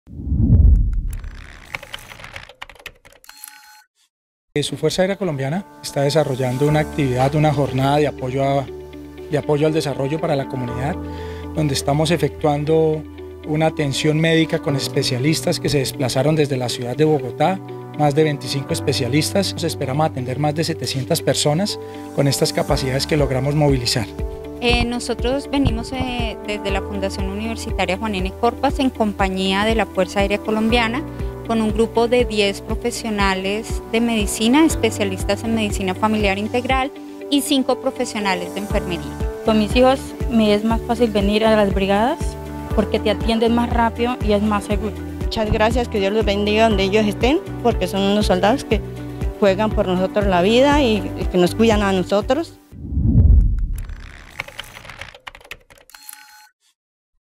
Jornada medica en Casanare organizada por la Fuerza Aérea - La Red TV Casanare
La Red TV Casanare - Marcamos la diferencia

2022©